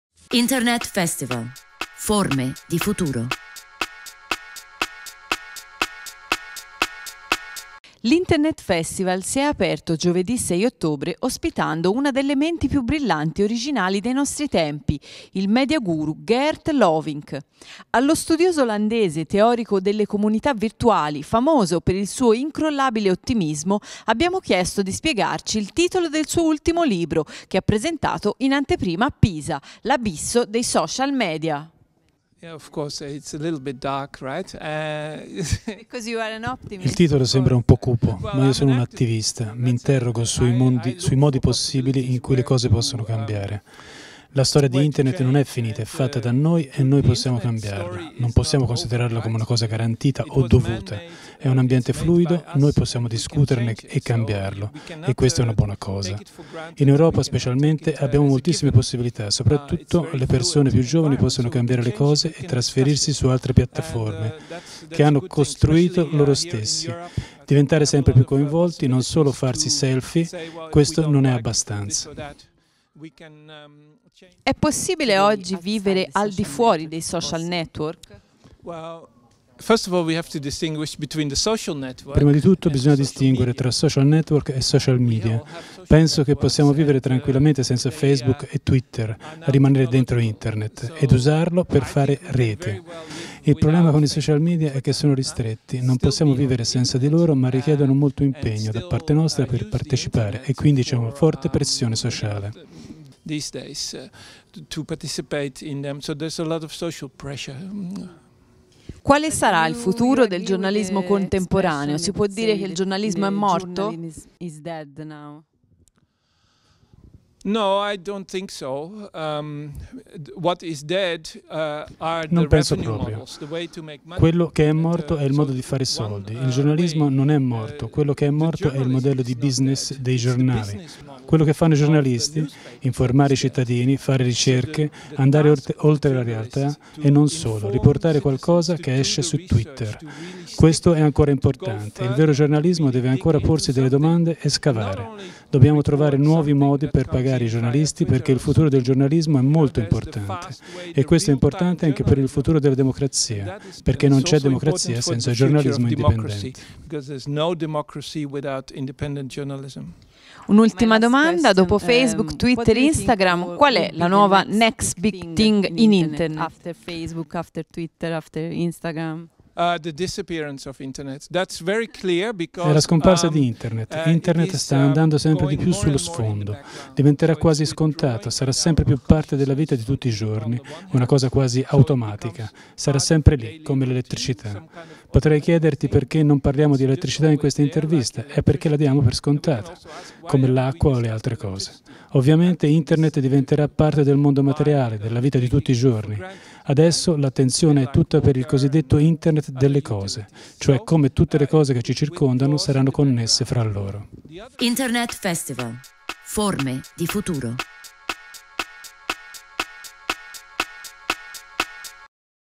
Qual è il futuro di Internet? Intervista a Geert Lovink
Il media guru olandese ha presenato in anteprima il suo libro "L'abisso dei social media" a Internet Festival